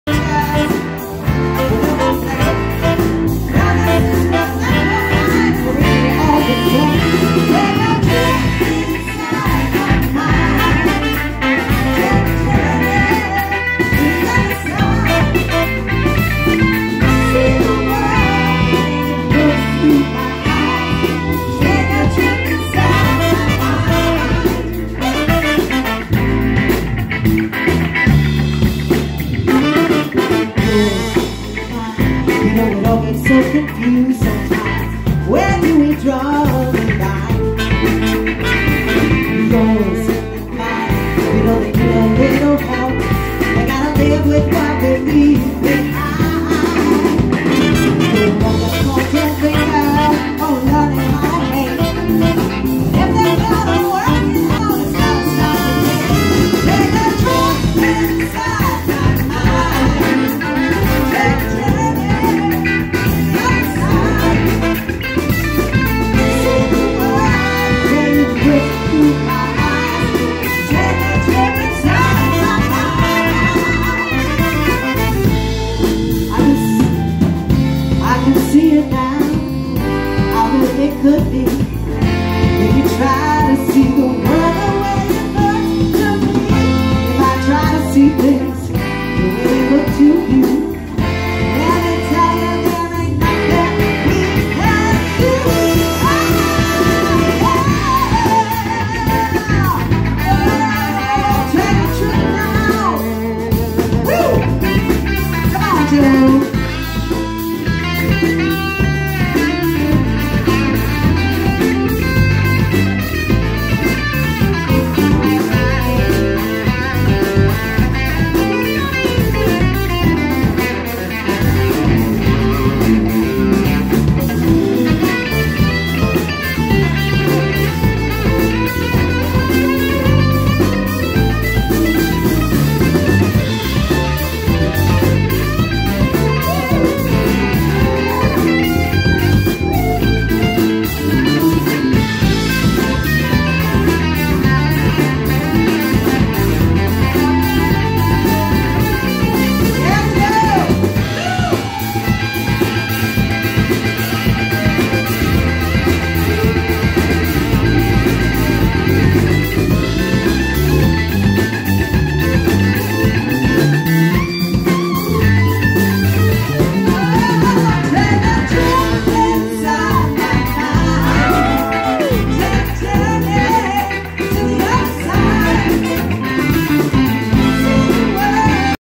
Some outstanding musical talent.
@JenniferTruesdale and @joesrockguitargarage along with a bunch of other fantastic musicians in Somerville MA.